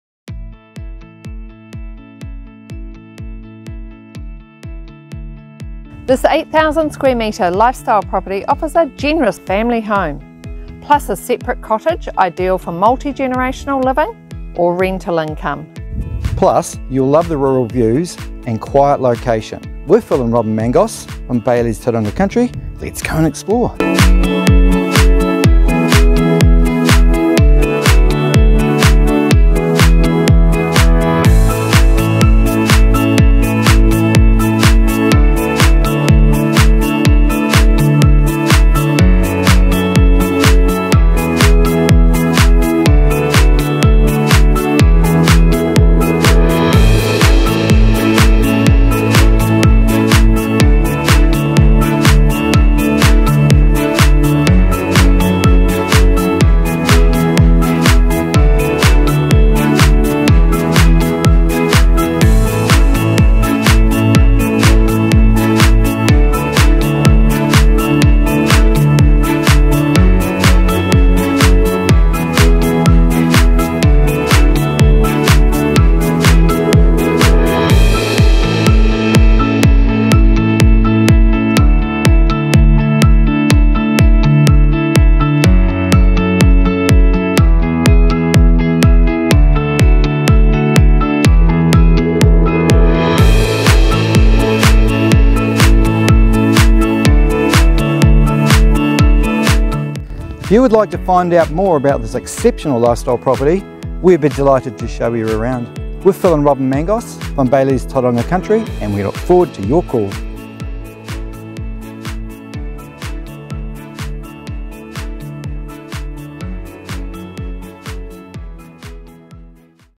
Nature nurtures a flexible life
Property: 7 Bonnieglen Road, Lower Kaimai (Bay of Plenty, New Zealand)

A hidden haven near the Omanawa Falls where two dwellings nestle in sunny seclusion. The main home enjoys a beautiful bush view while the cottage gets perfect privacy in the grounds.
This attractively established property benefits from the shelter of high hedges and the wide outlook over the native bush of the Omanawa Gully.
The main home was built in 2019, designed for all day sunshine and panorama. At 212 sqm (more or less) it is generous, yet the layout allows for intimacy if a couple are calling it home, with ample room for a family. Living areas (separate lounge and open plan area linked by the dining space) and the master suite face the views, attracting morning sun from the east while the afternoon sun floods the lounge via its west patio. Outdoor living spans the living spaces merging seamlessly to an in-ground salt-water pool, ideally sited for keeping an eye on frolicking children.
Black and white grounds a crisp bright ensuite, ingeniously linked to the main bathroom which echoes its décor and is easily accessed from the four family bedrooms.
It's both appealing and practical with features like interior wall insulation, economical underfloor heating and solar panels which keep the power bill down. Grounds are easy care and dotted with fruit trees. A three bay shed adds storage, a potting shed ideal for greenfingers.
A huge bonus to have, discreetly sited behind its white picket fence, a one bedroom cottage where you can fold back doors to the private patio fronting the living area and largest bedroom. The high quality of the interior invites options for accommodating parents, other extended family, guests or as a bed and breakfast. The beautiful new Omanawa Falls walking track some 900 metres away will be a drawcard for your visitors.
A peaceful place to retreat or share. While there may be bush as far as the eye can see you live conveniently close to civilisation, Tauranga Crossing retail centre approximately 15 minutes’ drive away.

Phil Mangos
Bayleys Real Estate
phil.mangos@bayleys

Robyn Mangos
Bayleys Real Estate